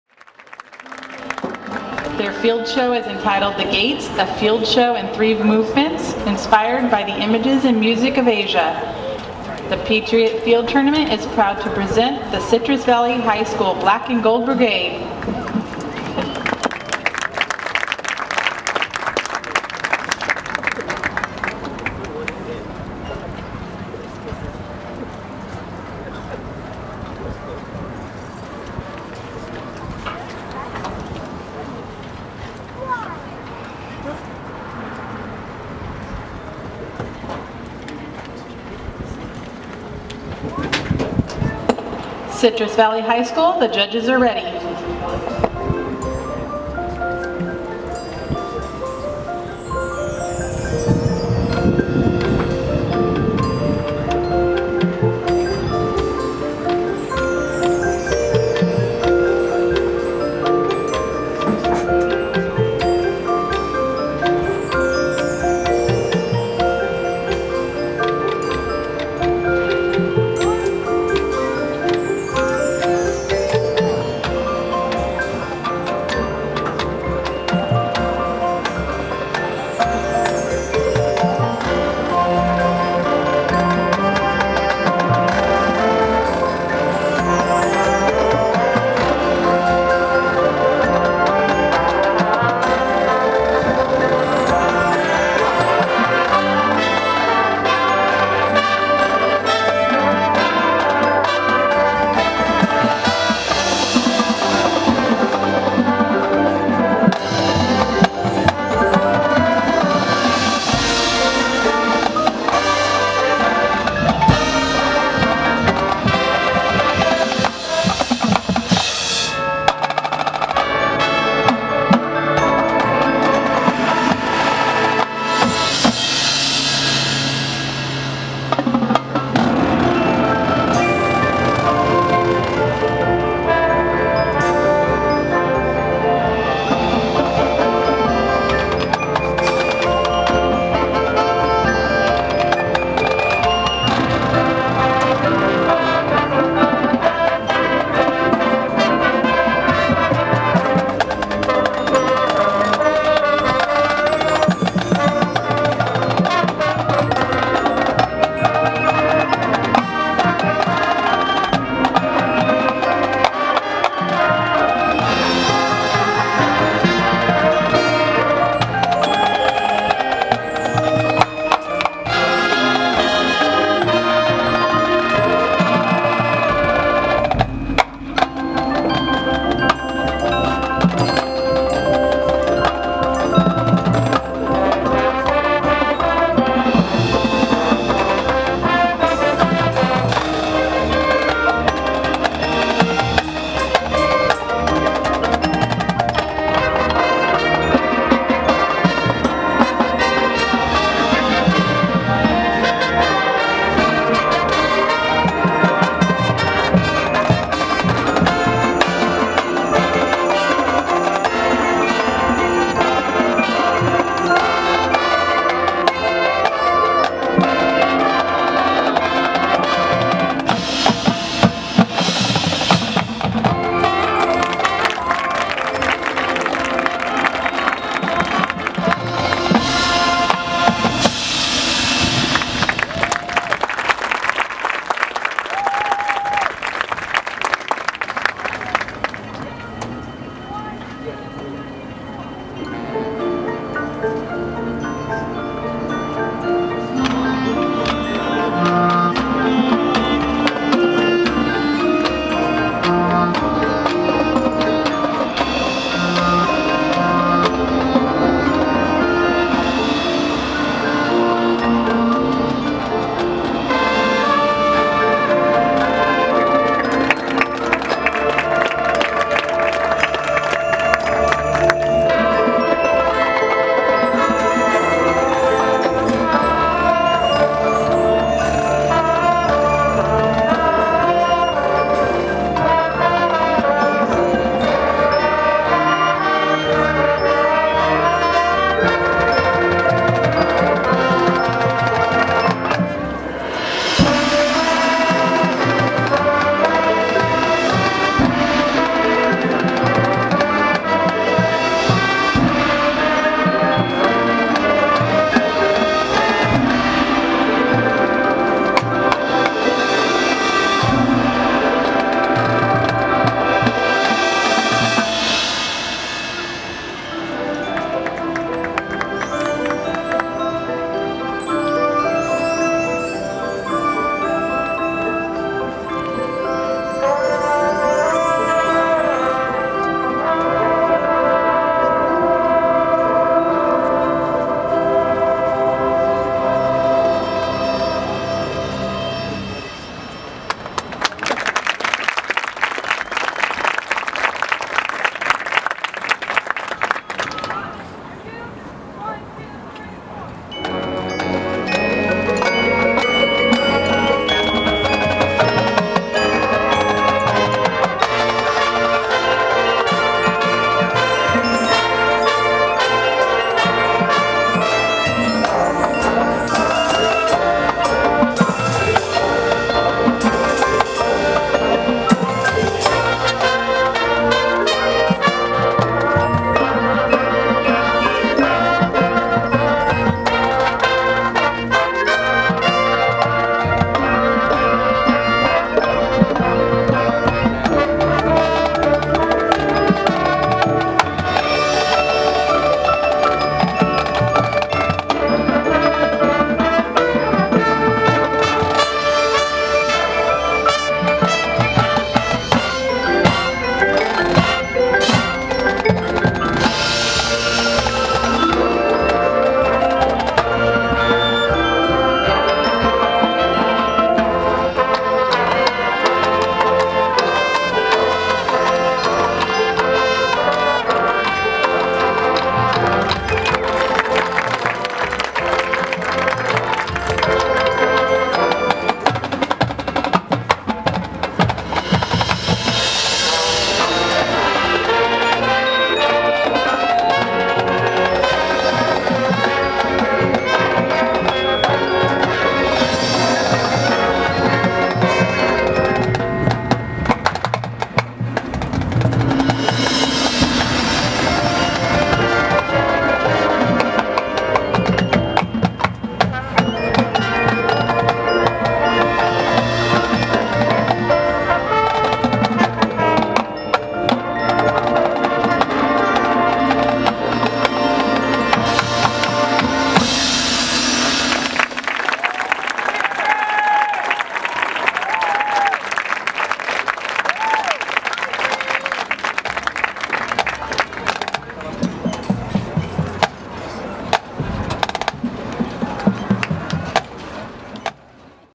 2013 Patriot High School Field Tournament Citrus Valley High School
The Citrus Valley High School "Black and Gold Brigade" performs at the 5th Annual Patriot High School Field Tournament in Jurupa Valley California.